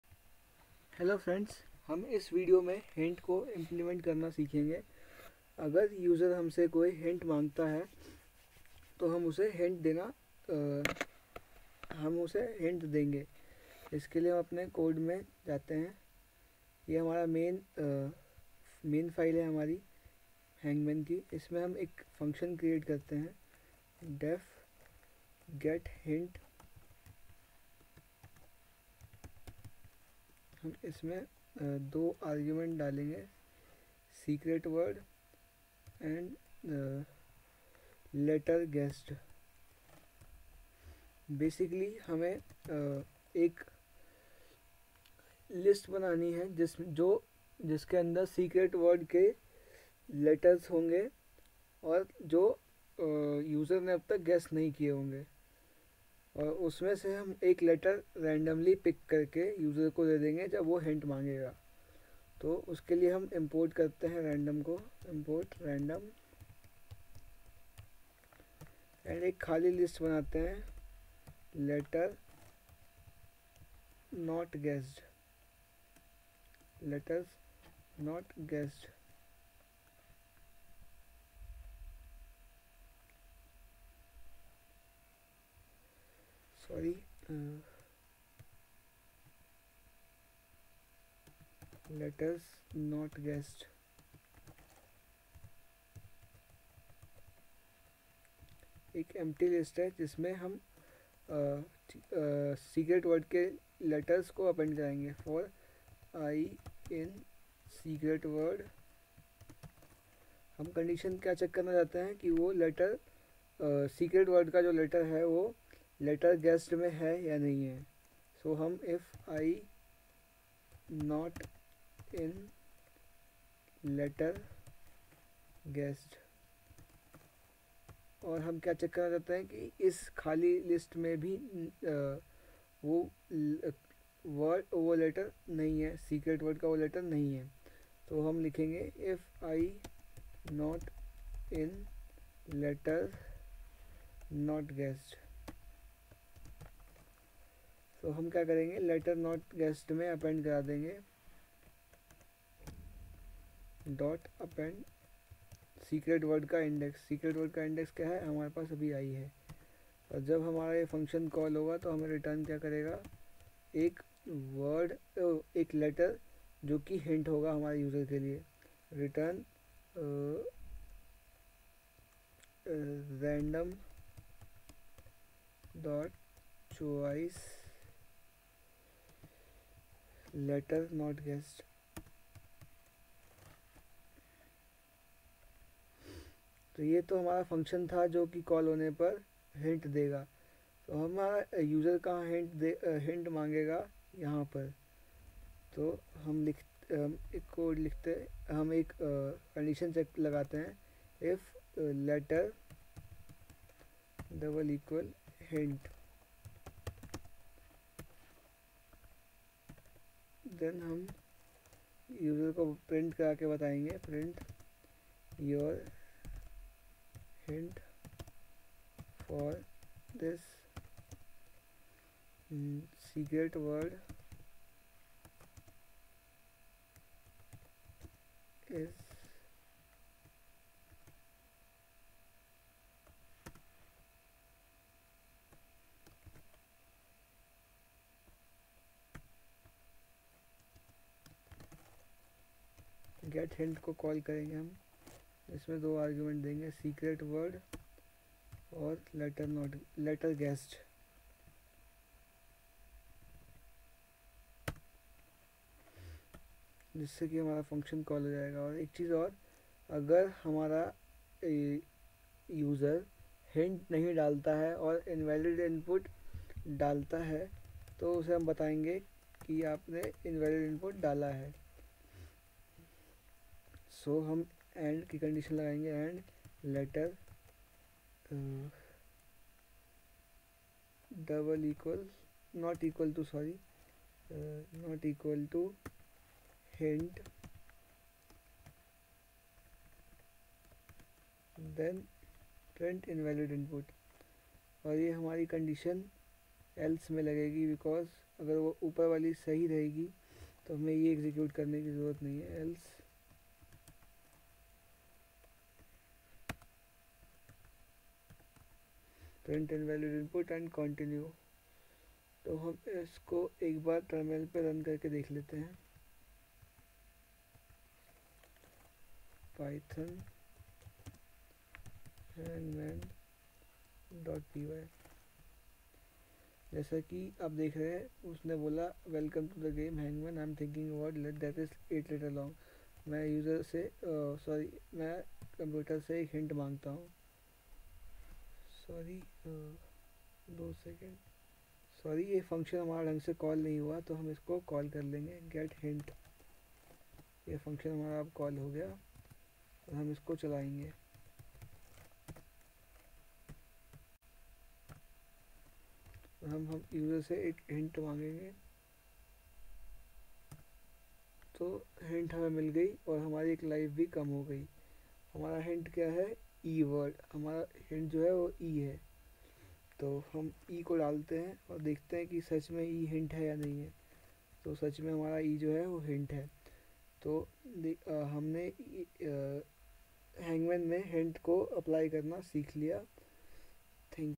Hint | Hangman
How to show the user some hint?

This video is made by Sanjiv Kumar (2018 Batch)